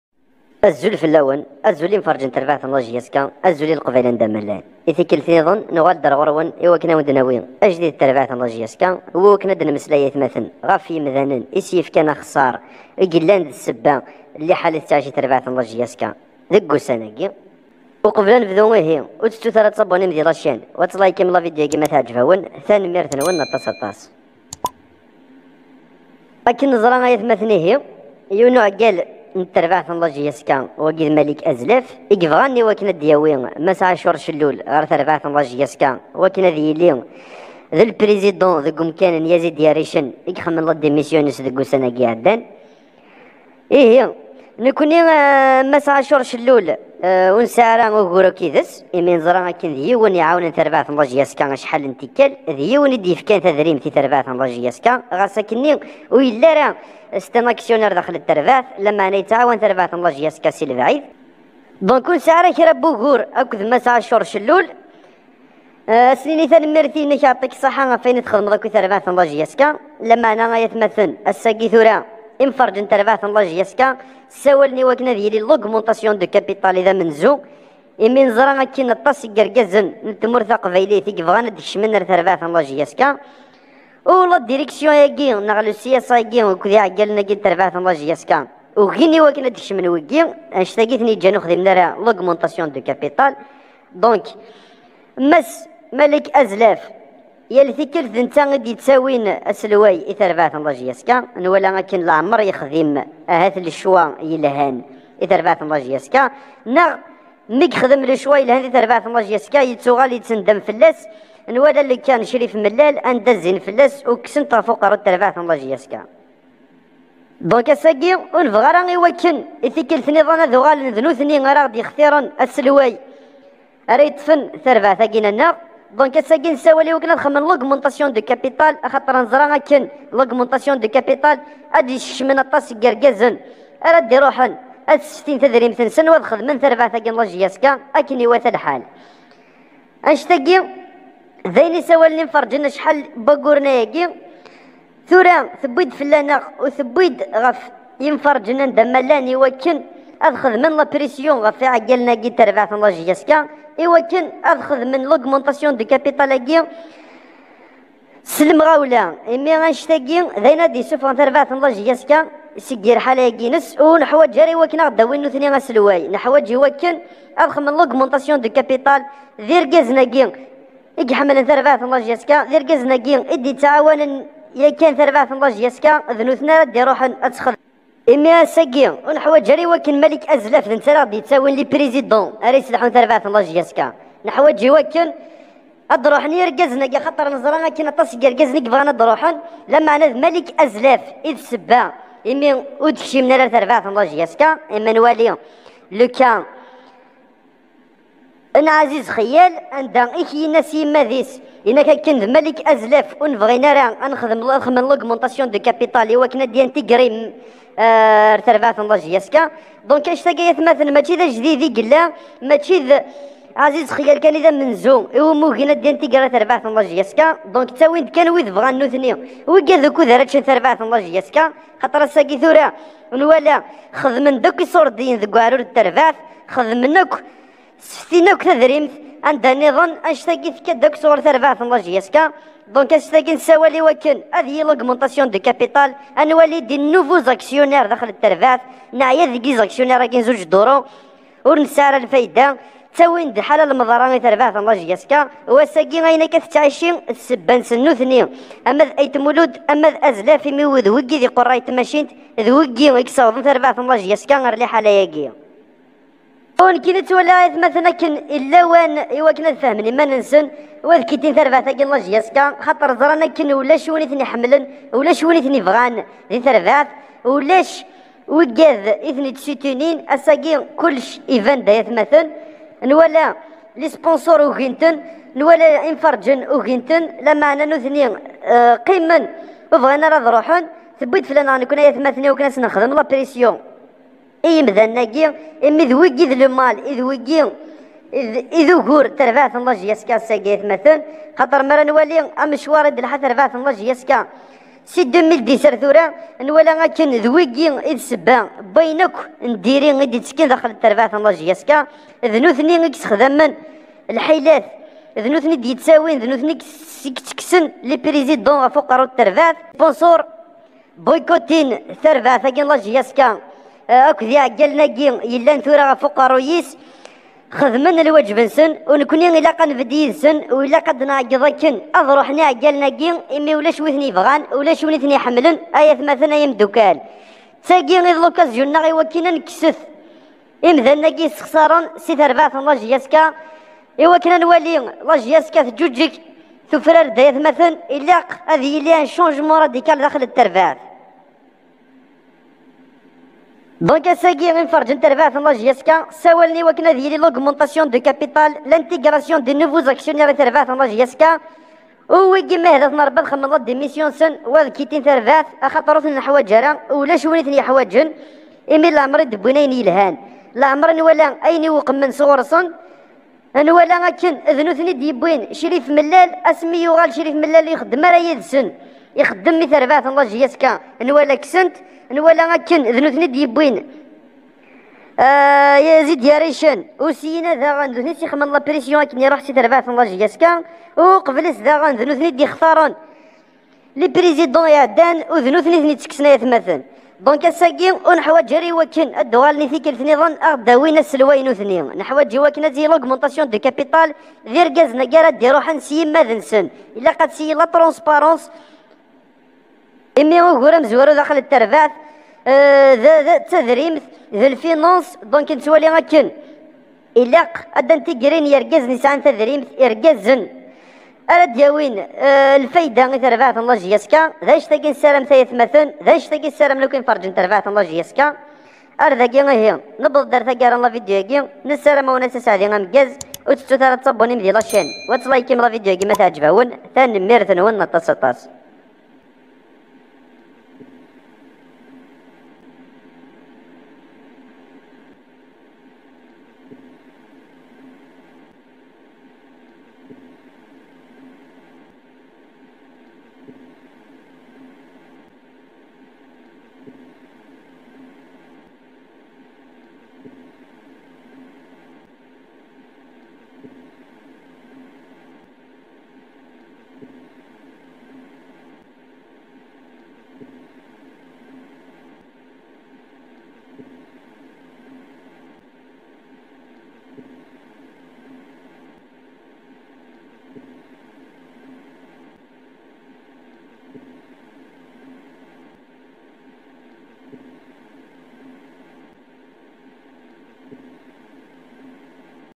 jsk Les Causes Des Problèmes De La JSKabylie
Les Supporteurs demande la démission de Azlef Est ait Mouloud Sur La tête de JskabylieLa nouvelle Direction de JSK avec Yazid Yarichene
Karim Ziani le directeur sportif de Jskabylie
Le match JSK la coupe du CAF
Le Problème Financière dans la jskabylie
La nouvelle Direction de JSK Avec un problème Financier